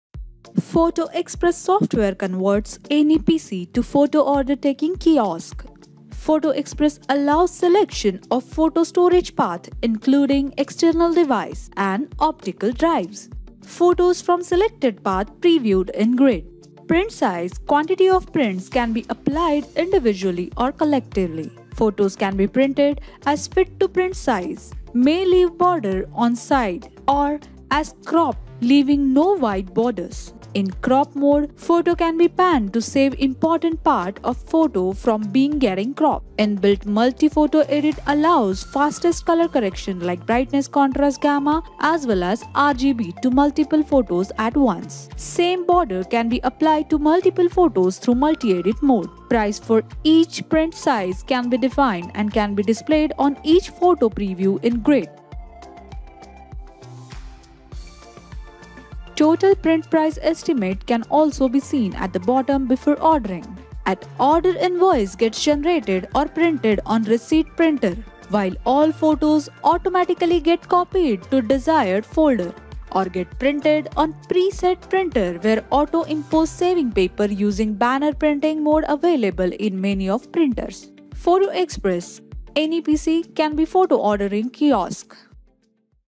Photo Xpress for Photo Order taking in Jumbo Pack by DgFlick
Photo Xpress

Photo Xpress is instant photo managing and printing software. Let’s see how it works.
The very first thing we will do is importing photos. Simply browse and select the photo folder. Photos in the selected folder will be displayed. Photos can be selected in one click. In case, you want to select all the photos for processing, use the ‘Select all’ option. Photos can be individually edited using the edit option available on each photo. Or, you can double click on photo to open it for editing. Similarly, multiple photos can be opened for editing at a time using the 'Edit Photo' option.
Here we can see that multiple photos are opened for editing. On the right side of the window, you get photo enhancing options. Color Correction tab has color correction tools that can be used with sliders or value boxes. You can manage the brightness, contrast and gamma, Red, green, and blue values of the photos too. Corrections can be individually reset or reset completely with respective reset options.
Photos can have a title on it. Navigator is given to navigate between selected photos. Digital frames can be added to the photos from the Border tab. Select a photo and apply the border by double-clicking on it. Tools like flipping and rotating are available for the applied border. You get several cliparts of different themes that can be added to photos. Add desired clipart by choosing a theme or browse and get your own clipart on the photo. You may generate ready tones of applied clipart and use any of them.
You can add some effects like colorize, blur, etc. on photos. In each effect, you will find some presets that can be applied and managed from the Custom panel. A photo can be resized in the size you want to print. Select the size you want to print. In addition, quantities to be printed can be defined in advance. The photo in the printing size can be fitted or cropped. In case, you cropped the photo, you may lose some part of the photo depending on the printing size chosen by you. In case, you fitted the photo, you may see some white space around the photo. A photo can be rotated by 90 degrees clockwise and counterclockwise from respective options. You may rotate the photo slightly with the plus or minus signs. The photo will keep on rotating on each click. The rotation angle can be seen here. The rotation you applied through plus and minus signs can be reset from the Reset option. A face area tool is given in case you want to occupy a certain part of the photo with the face. To display the face area guideline, check the Show checkbox. The percentage of the face area can be set from here. The color of the guideline can be changed and color which gives maximum visibility on the photo may be chosen. Photos can be either directly printed on a connected printer or saved as JPG.

This is how you can edit your photos quickly only in Photo Xpress.